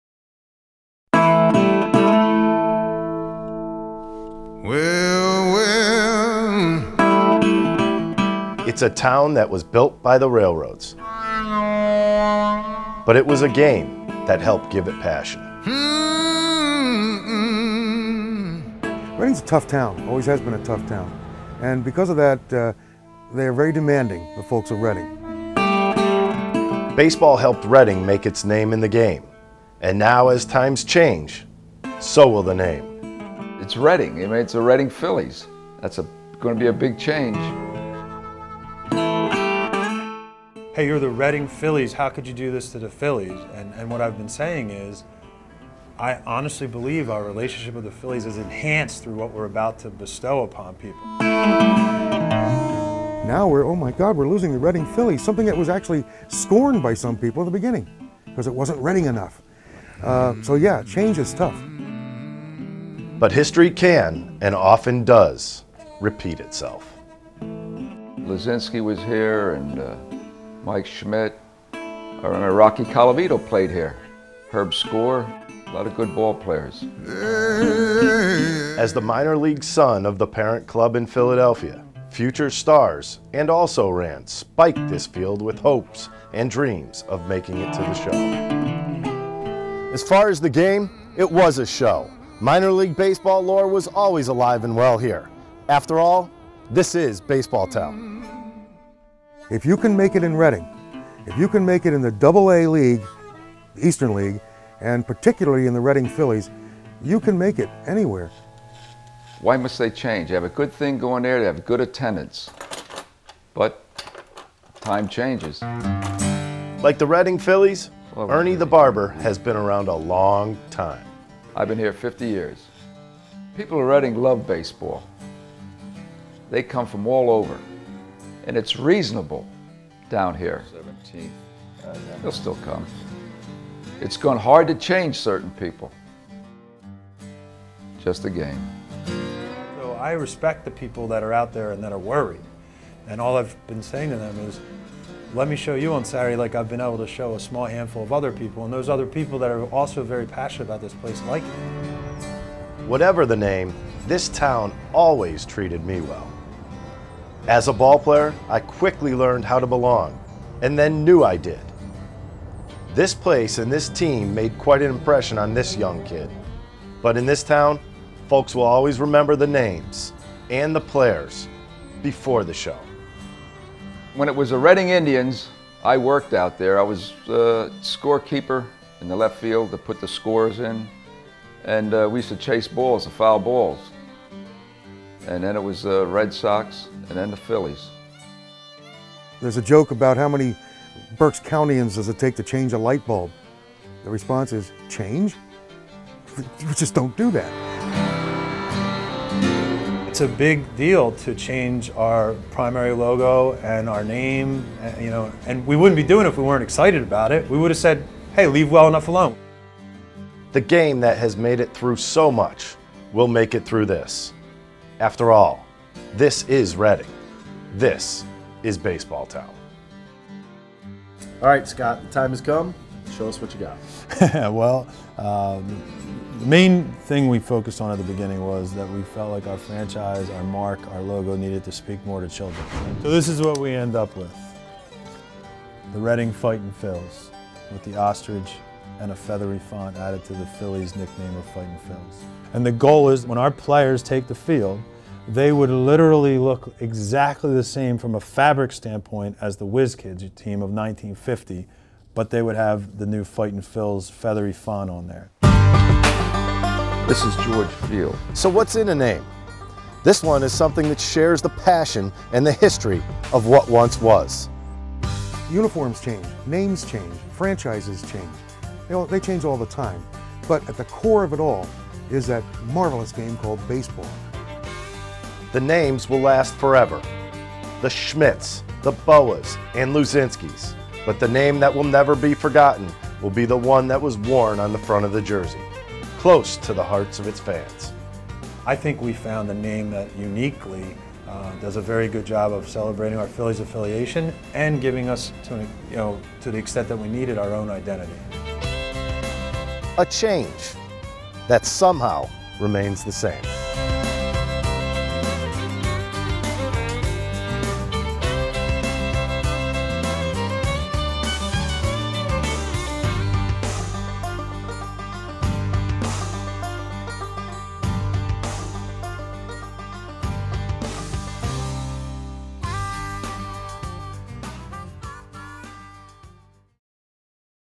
A Team, A Town and A Name
"Professional sports is filled with change...players, coaches...signed and fired...we've grown used to that sort of off filed action.  But when the change affects  what fans could feel is the  heart and soul of a team...that's when things get sticky.   After being called the  Reading "Phillies" for nearly 50 years, that franchise has decided to make a change. Former Reading Phillies pitcher Ricky Bottalico went back to Reading to take the pulse of change with the town, the team and a  name."